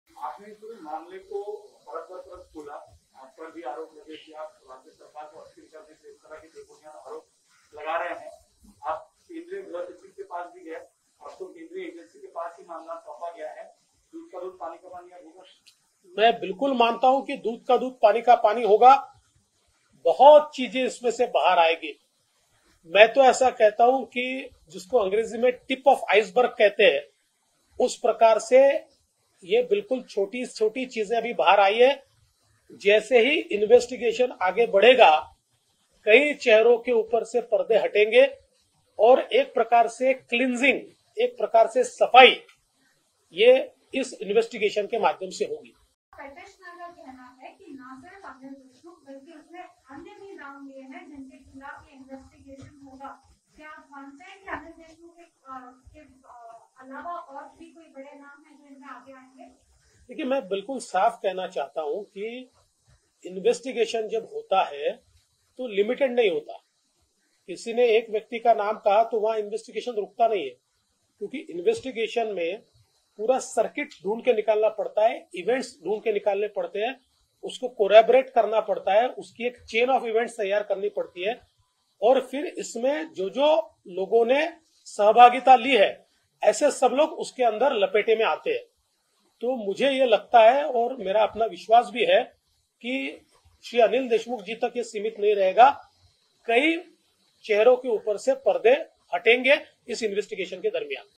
कई चेहरों सें अभी पर्दे हटेंगे...
This is just the tip of the iceberg..
(प्रेसवार्ता, मुंबई । दि. 5 अप्रैल 2021)
- LoP Shri Devendra Fadnavis